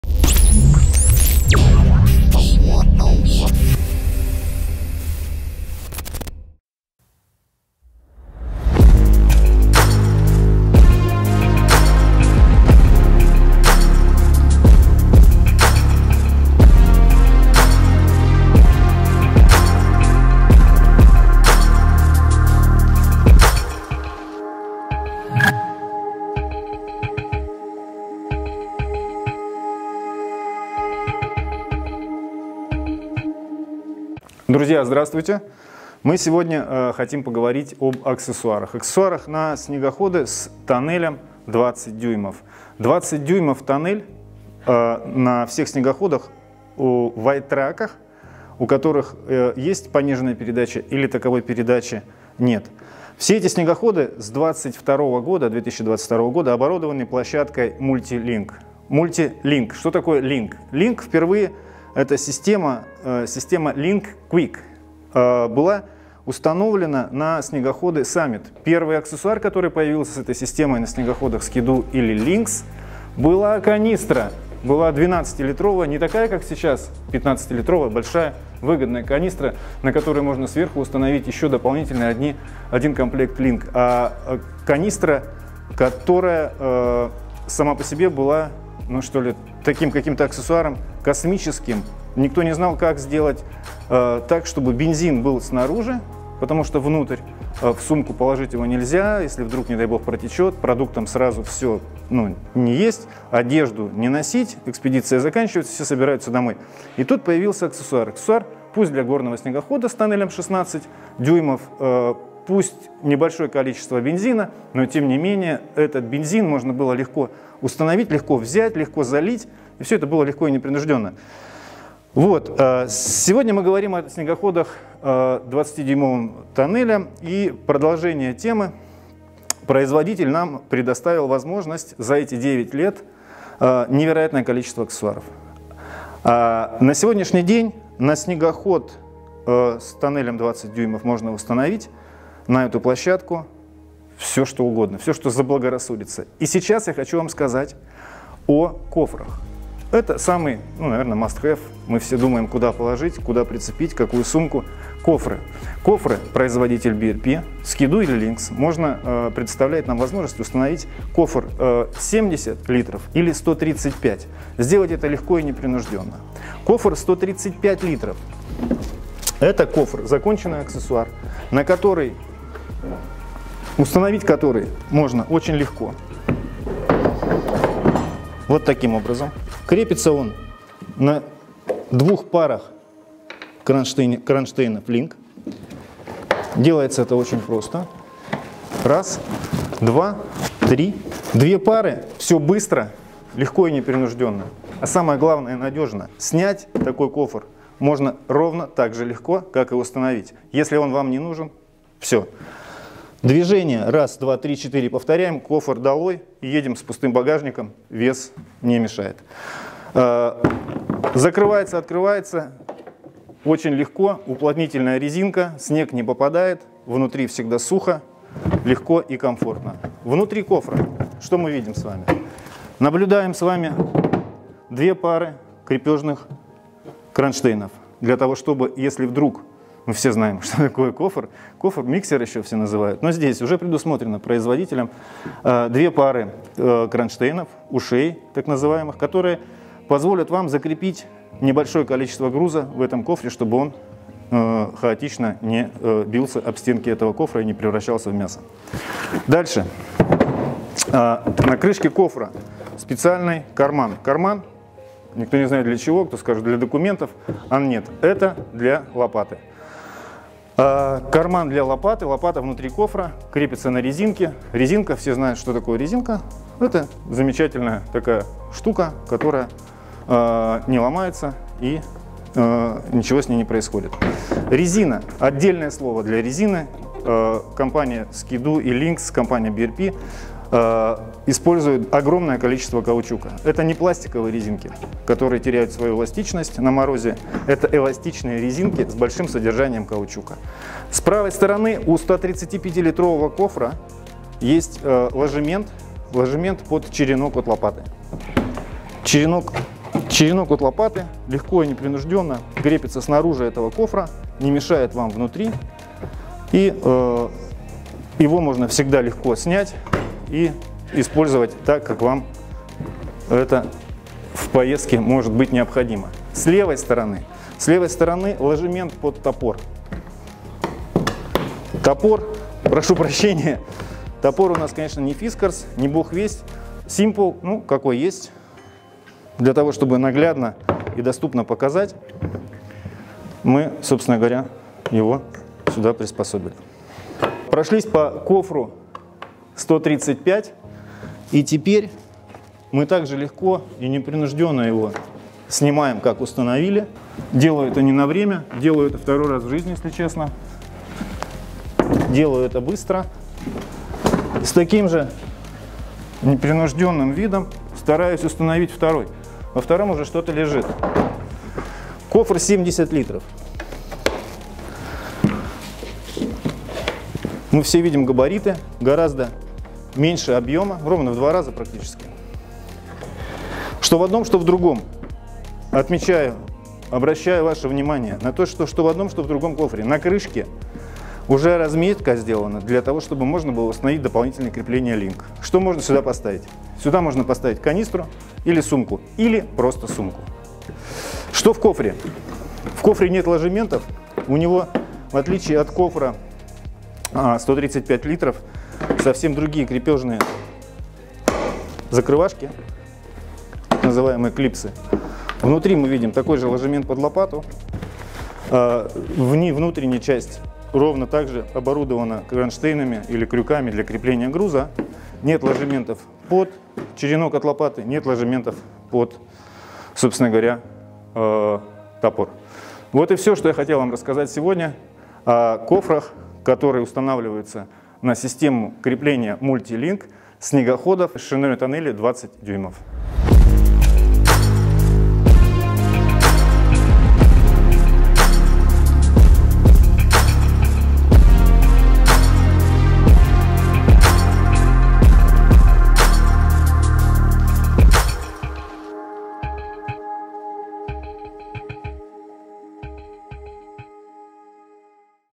Платформа LinQ и багажные кофры 135 л и 70 л - аксессуары LinQ для снегоходов Ski-Doo и Lynx
Выпуск № 1 - система креплений LinQ и навесное оборудование для снегоходов Ski-Doo и Lynx - багажный бокс 135 л и 70 л.